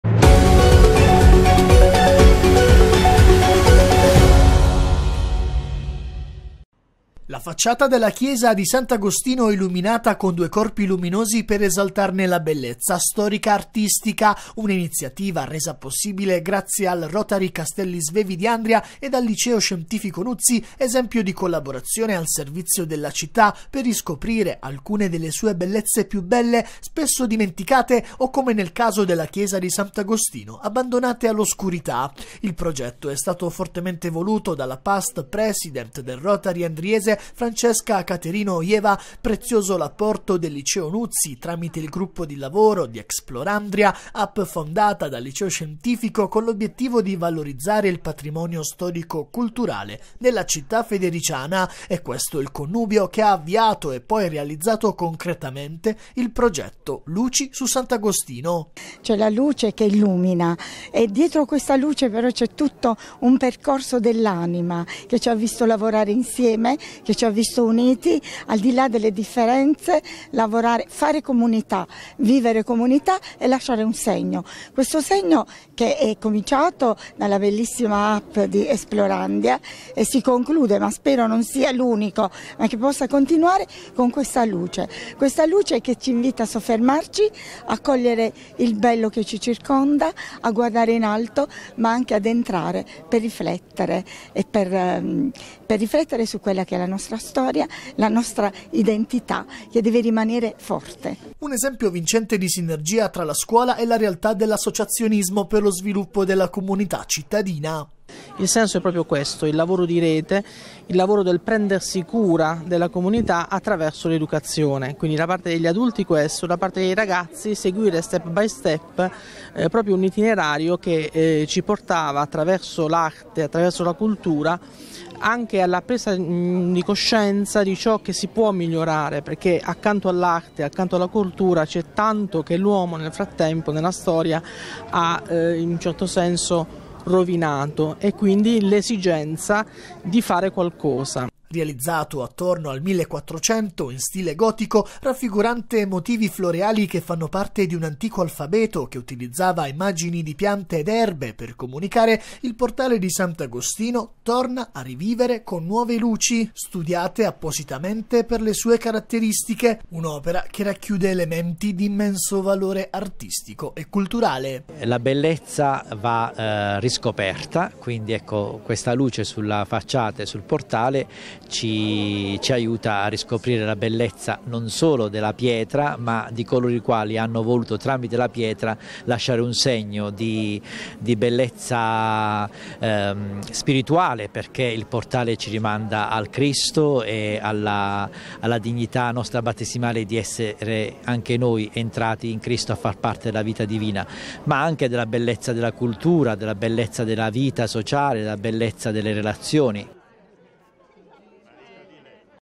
ANDRIA | Rotary Andria e Liceo "Nuzzi" illuminano la facciata di Sant'Agostino
Nuova luce su storia e arte della città